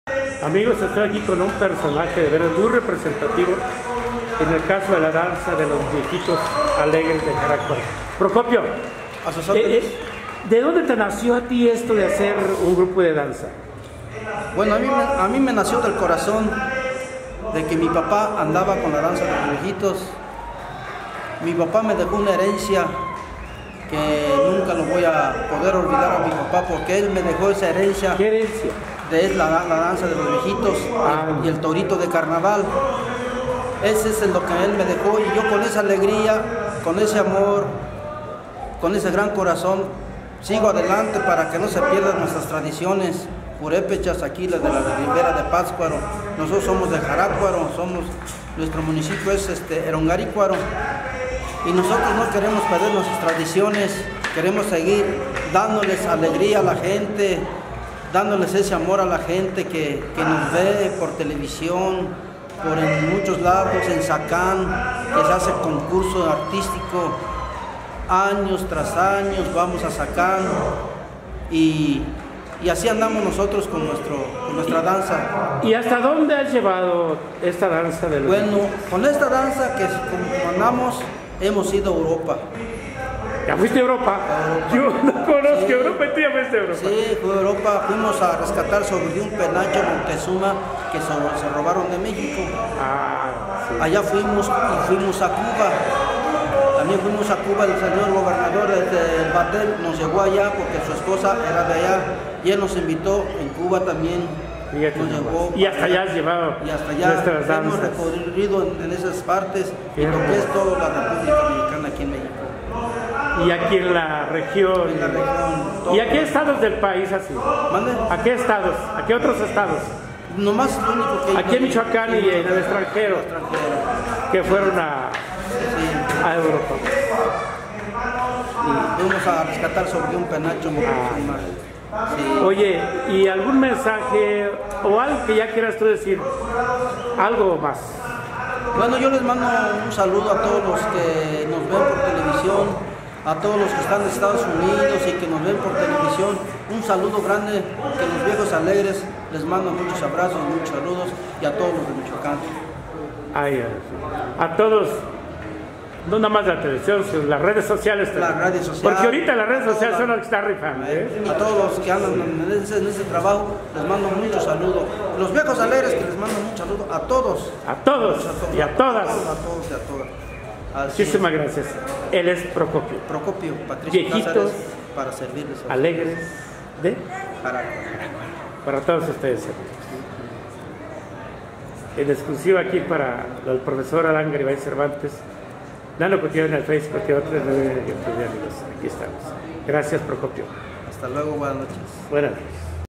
Charla con el Director de Los Viejitos Alegres, Concurso Tradicional Purhépecha, Caltzontzin 2018
UnA charla con el Director del grupo de danzantes y Orquesta de Los Viejitos Alegres deJarácuaro, durante el Concurso Tradicional Purhépecha  realizado en la Tenencia de Caltzontin, municipio de Uruapan del Progreso, Michoacán el día 5 de julio de 2018, con motivo de fiesta patronal en honor del patrono San Salvador Cambutzio.